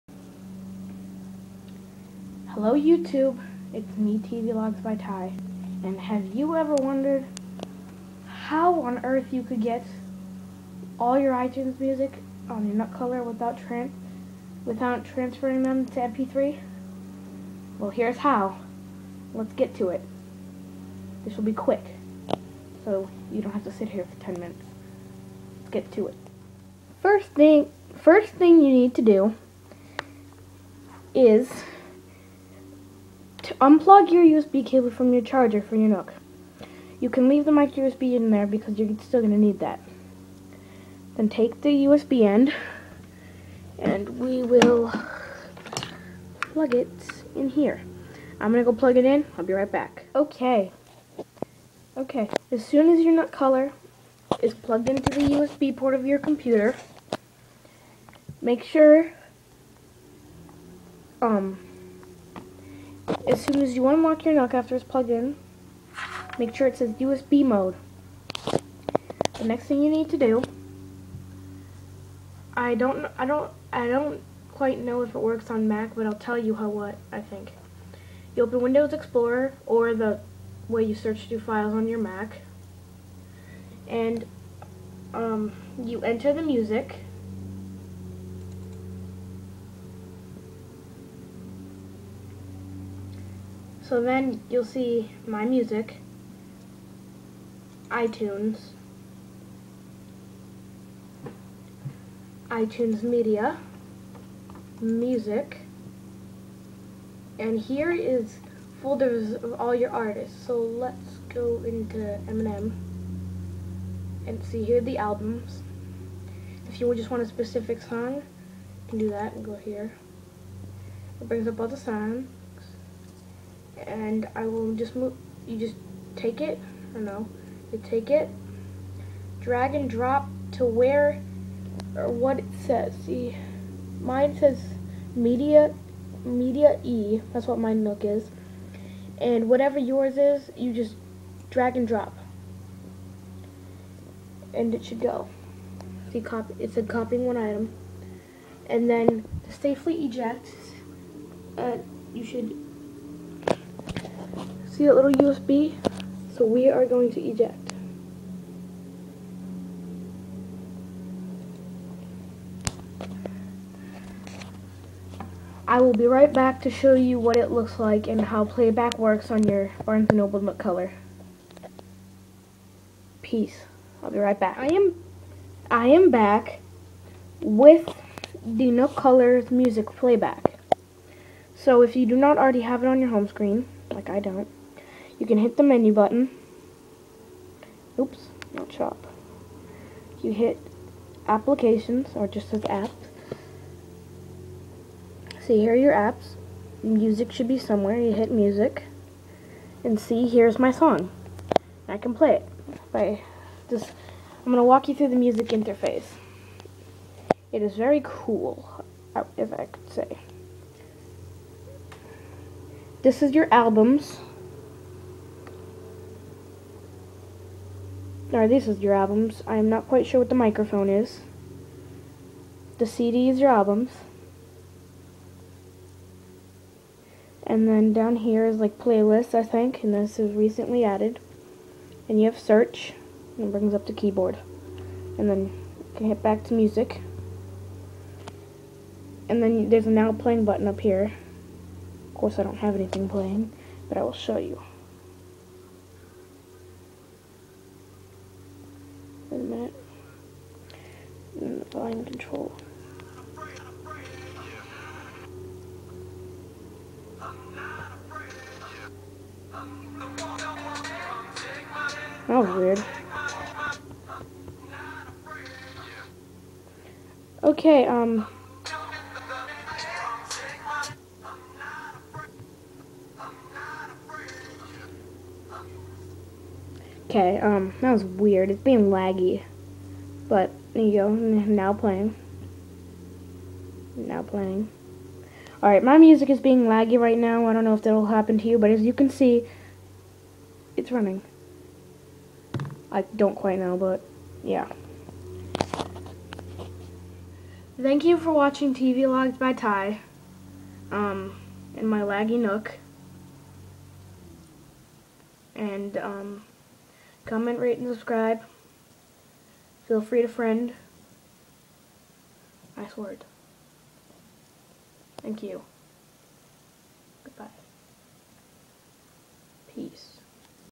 How To Transfer iTunes Audio To Barnes And Noble Nook Color
How you ever sat down and wondered how to get the thousands of songs in your iTunes library onto your newly unboxed nook color? Well here is a way to do this without rooting, transferring to mp3, etc., You can now do this in under 5 minutes!

Filmed in 720 p HD movie on my iPod touch and edited with Apple IMovie.